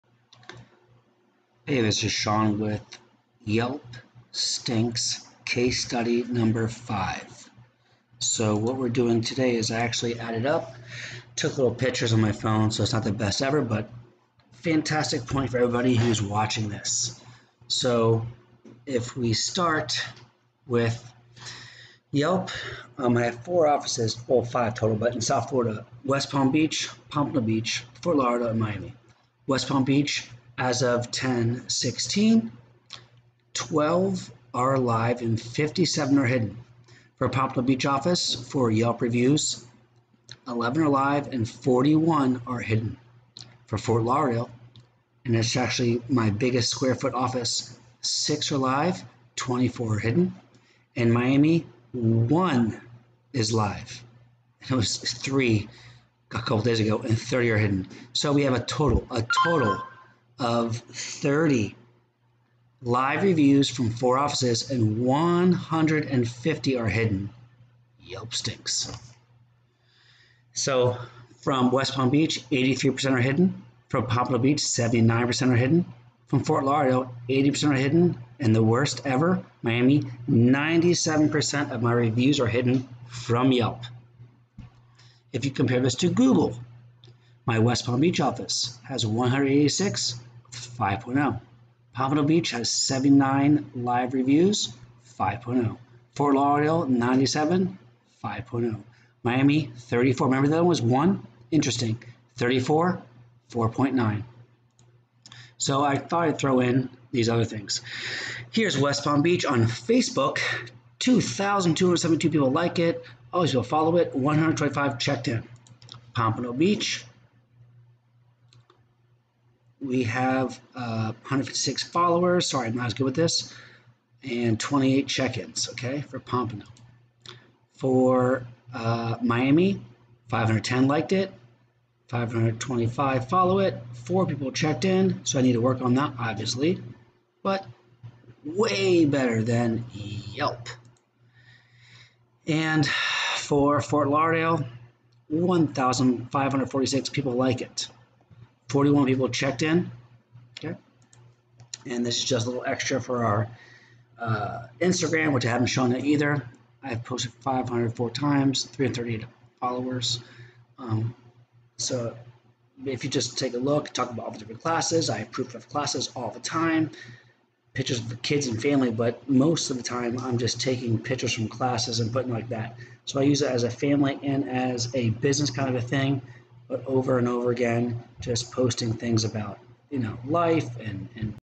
Yelp Stinks Case Review 5
In this video, we show the percentages of our offices that are hidden on Yelp compared to what we have on Facebook, Google and Instagram. Yelp HIDES reviews on purpose!  This one is good and eye opening. As always Yelp Stinks!

It is crazy that one of our most popular offices, Miami, as of Oct. 17th has only 1  (one) UNO review live!  Amazing!  Yelp is such a scam.  We have over one thousand likes on facebook and hundreds of check ins for the same office on facebook and instagram.  Yelp is hiding this office to make more money off of other offices near me for their same $

Everyone knows CPR Florida is the best.  The funny thing is we still come up #1 on Yelp for our office without paying, but they still try to hide it.  We will see if after posting this video they try to make our ranking go down.  If they do, we will have even more proof if or when we need to go go court.  Can't wait :)
From the awesome staff of 22 strong at CPR Florida.  Always #1 in everyone's eyes (except yelp)